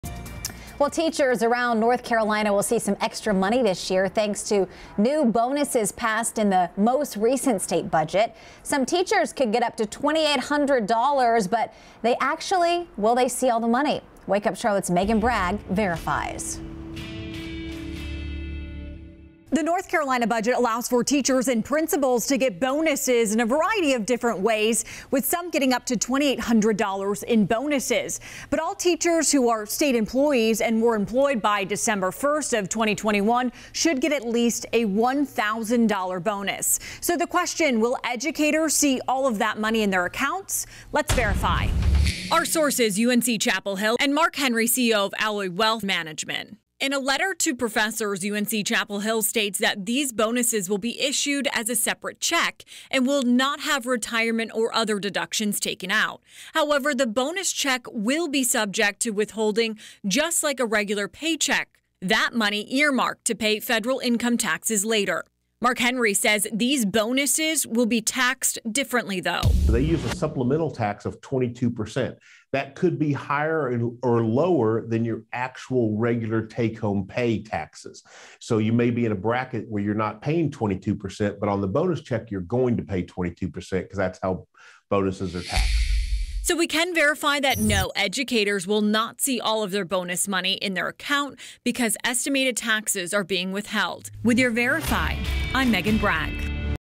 No, educators will not see all of their bonus money in their accounts
The North Carolina budget allows teachers and principals to get bonuses in a variety of different ways. Some educators are getting up to $2,800 in bonuses. However, all teachers who are state employees and were employed by Dec. 1 of 2021 should get at least a $1,000 bonus.

In a letter to professors, UNC-Chapel Hill states that these bonuses will be issued as a separate check and will not have retirement or other deductions taken out.

However, the bonus check will be subject to withholding, just like a regular paycheck, that money earmarked to pay federal income taxes later.